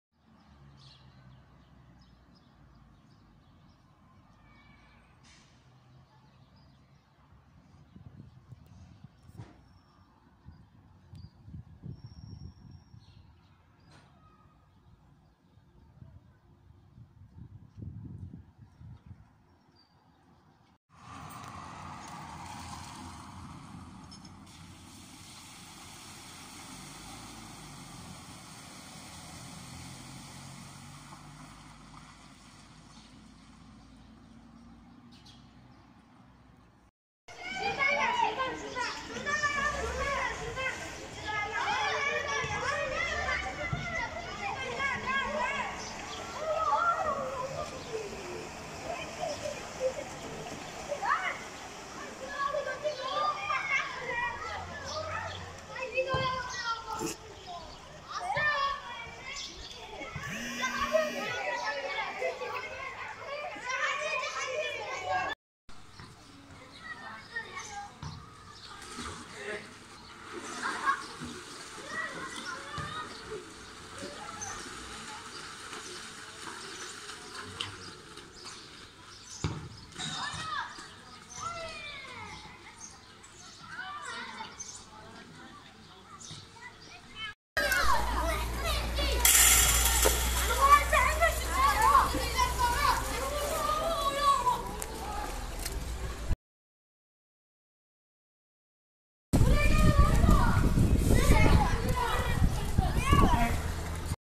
Apartman sakinleri biriken yağmur sularından tedirgin
Kahramanmaraş’ta yol üzerinde biriken yağmur suları yüzünden istinat duvarlarında hasar ve evlerinde su sızıntıların oluştuğu belirten apartman sakinleri, yetkililere yaşanan sorunun çözümü için çağrıda bulundular.
Onikişubat İlçesi Abdulhamithan Mahallesi, Katip Çelebi Caddesi üzerinde yer alan Ahmet Bey Sitesi önünde bulunan yol üzerinde sağanak yağış sonrası biriken suların tehlike oluşturduğunu ifade eden apartman sakinleri, cep telefonu kamerasıyla yaşananları görüntüledi.
Çeşitli tarihlerde kaydettikleri kamara görüntülerini ilgili kurumlara iletmelerine rağmen sonuç alamadıklarını iddia eden apartman sakinleri, su birikintileri yüzünden istinat duvarlarında ve giriş katlarda su sızıntıları yüzünden hasar oluştuğunu söylediler.
Yolu kullanan sürücülerin dahi güçlük çektiği kaydeden vatandaşlar, su birikintileriyle oynayan çocuklarının sağlıklarının tehlikede olduğunu dile getirdiler.